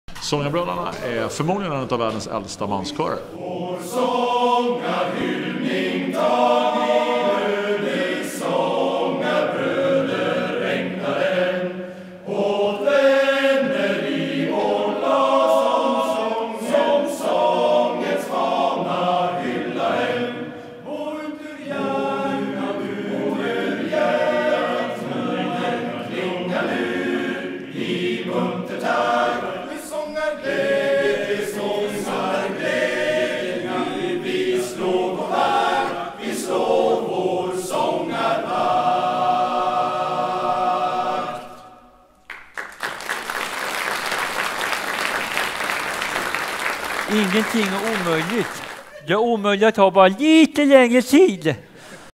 Kom och sjung i manskören!
Är du man, boendes i närheten av Boxholm och vill sjunga i manskör? Måndag 12/8 klockan 19 startar vi höstens aktiviteter på Nygatan 9.